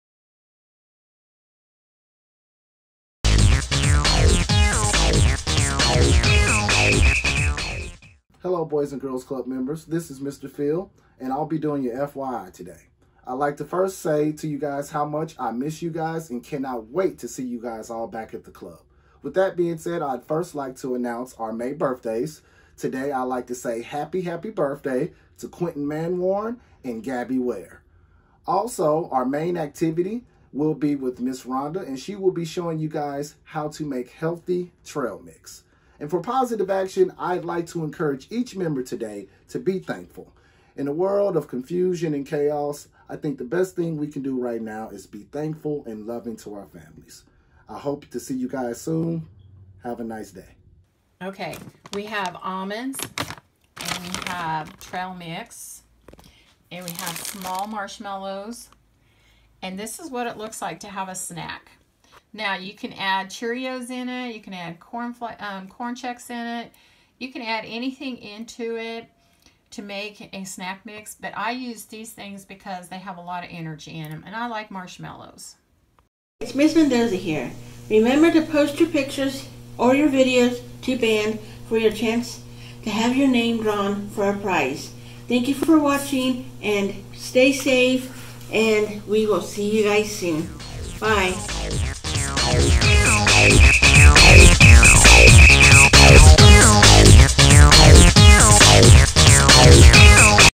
Club At Home: May 11
Today's video is short and sweet. It's Make it Monday and Ms. Rhonda shows us how she makes her favorite snack mix.